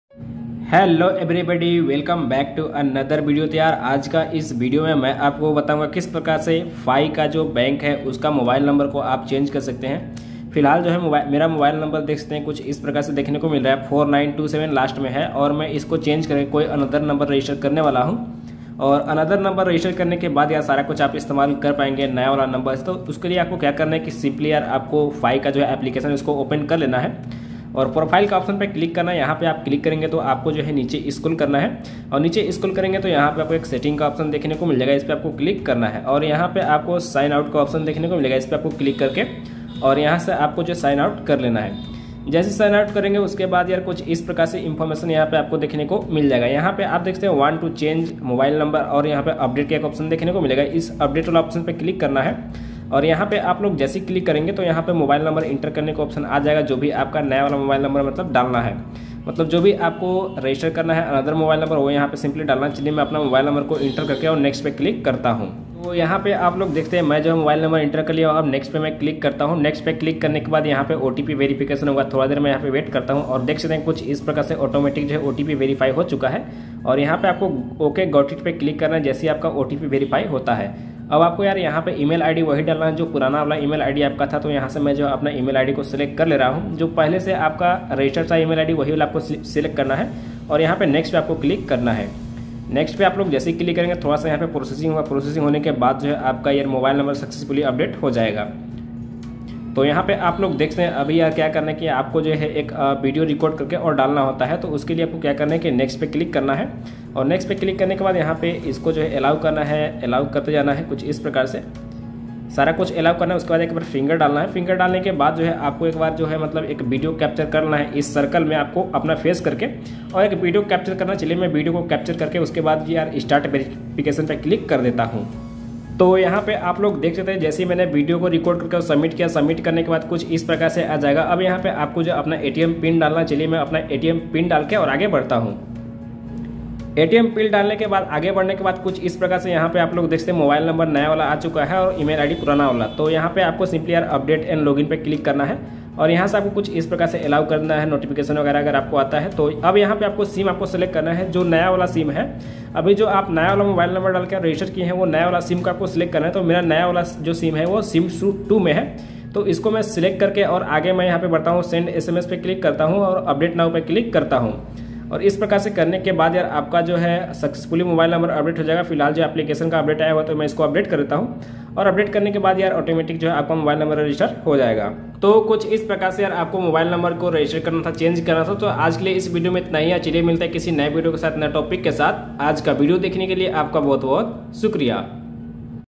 Change fi bank mobile number | Fi mobile number kaise badale
How to change fi phone number, FI बैंक मोबाइल नंबर कैसे बदले|
FI mobile number change process,
Fi mobile number.

00:00 Change phone number fi.
01:09 FI Mobile number badale.
02:21 FI bank sim change.